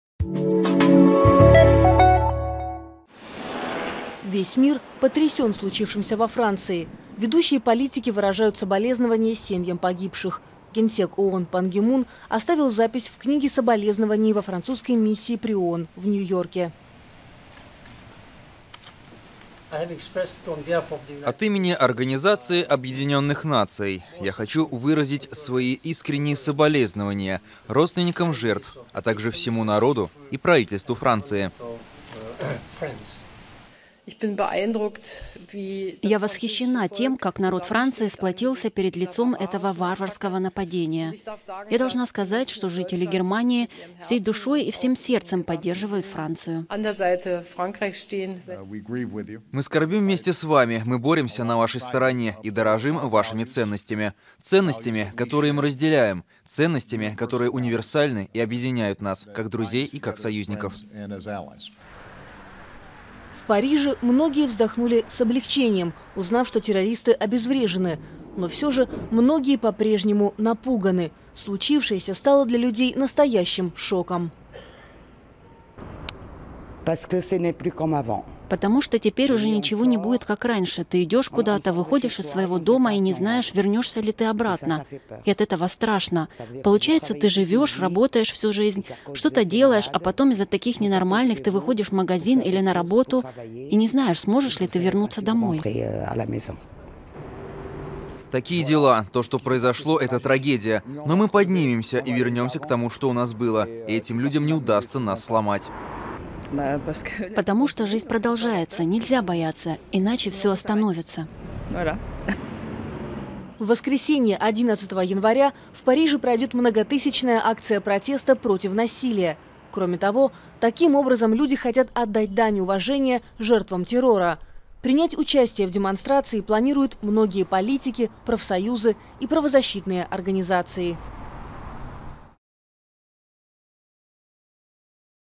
Франция после терактов: весь мир потрясен случившимся
Лидеры ряда стран выражают соболезнования семьям погибших в результате терактов. Ангела Меркель, Дэвид Кэмерон и Жан-Клод Юнкер в воскресенье собираются принять участие в многотысячной демонстрации против террора в Париже.